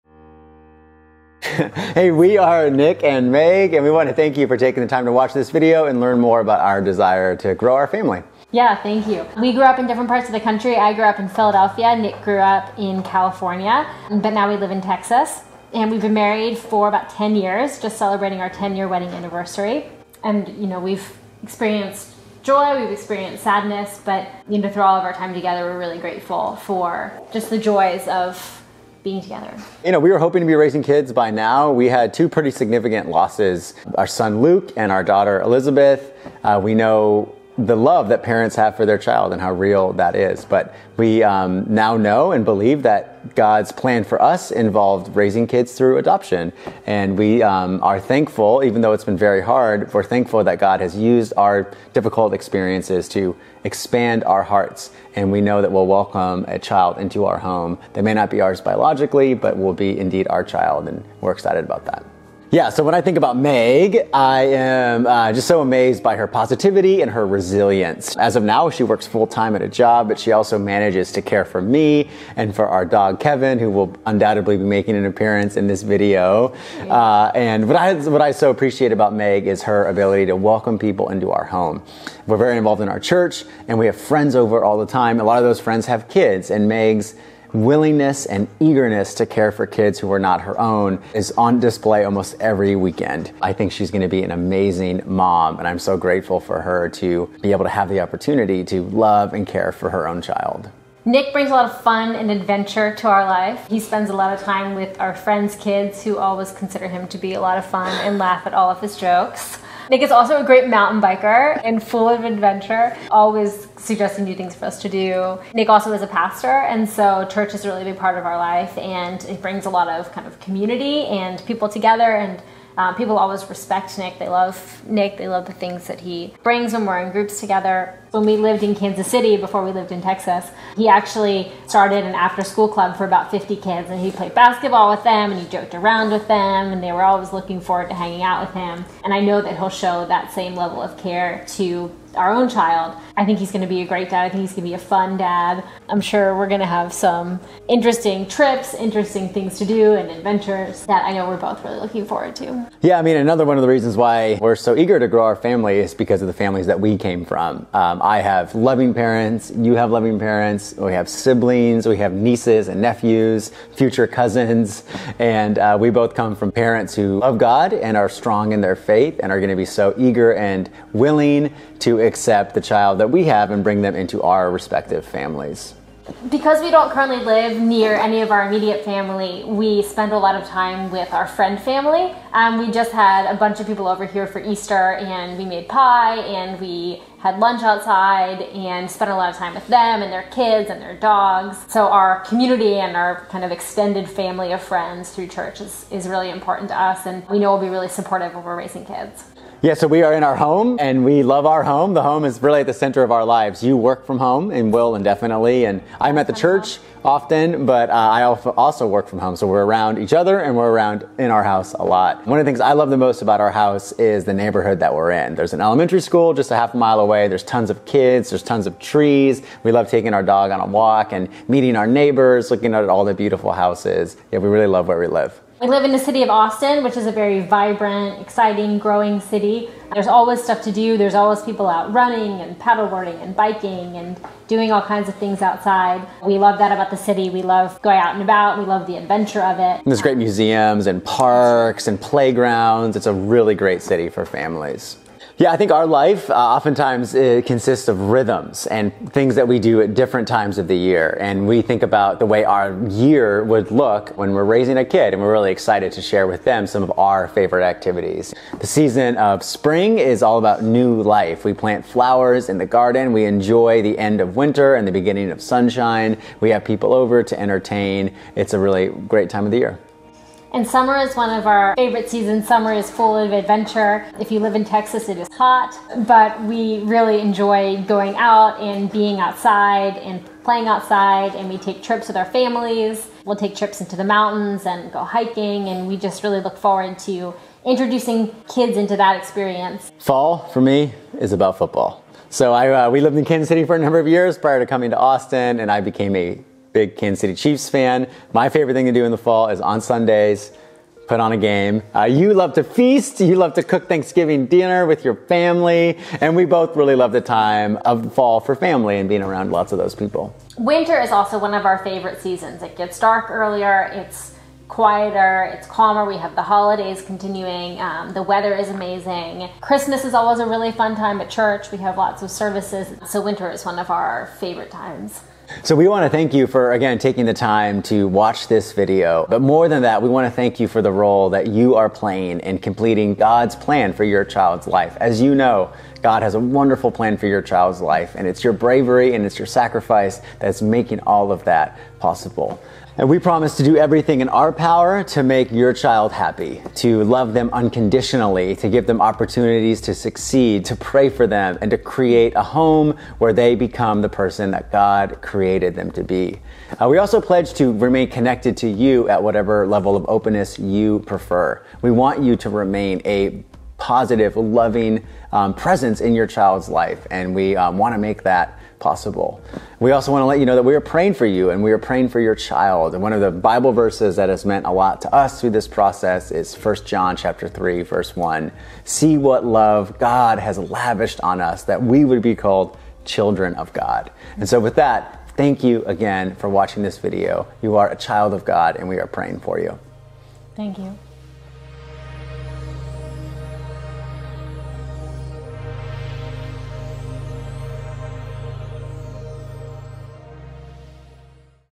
Nick and Meg
This video was made available for mothers choosing adoptive parents for their children. Thankfully, one mother who saw this video ultimately decided to allow Nick and Meg to adopt her baby. It is so rewarding to know that my work has changed people's lives for the better. Nick and Meg are now proud, loving parents to their child.